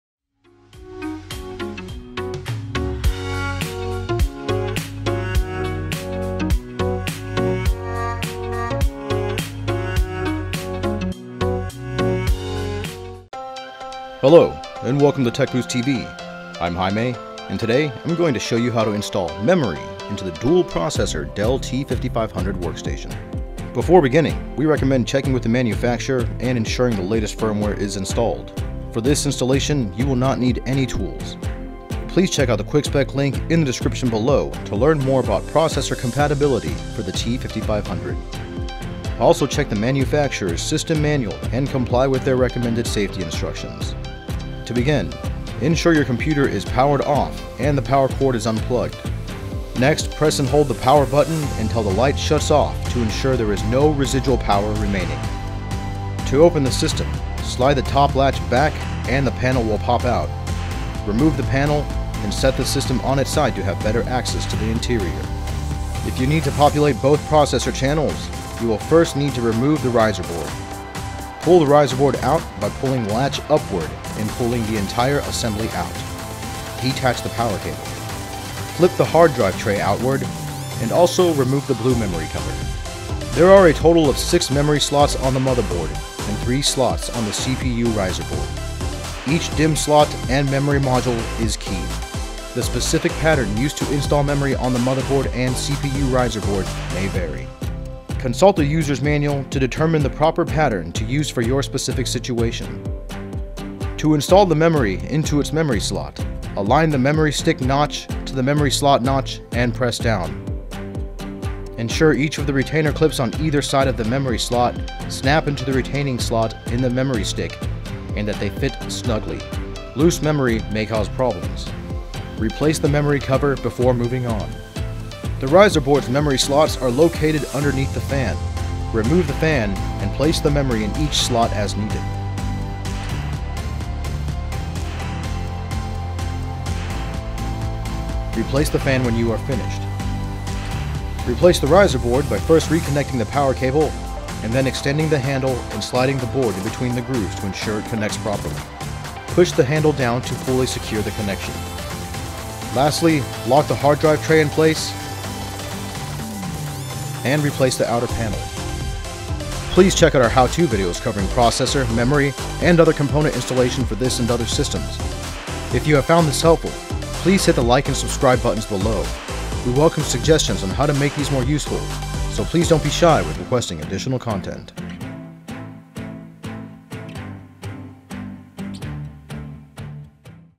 Dell T5500 Workstation Memory Installation Guide
Spec Sheet for Dell T5500 In this video, we will teach you the proper way to install memory into your Dell T5500 Workstation.

For more information and great on Dell and HP servers, workstations, and laptops, please